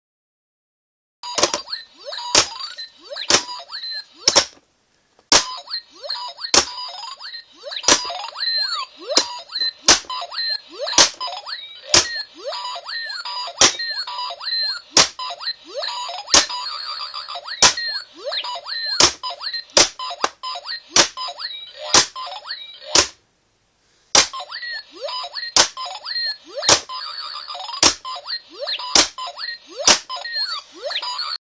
Młoteczek i łóżeczkowe robaczki - zabawka
Zabawka przybijanka z dźwiękami. Młotkiem wbijamy robaczki do łóżka, a przy tym robaki wydają zabawne dźwięki i łóżeczko się podświetla.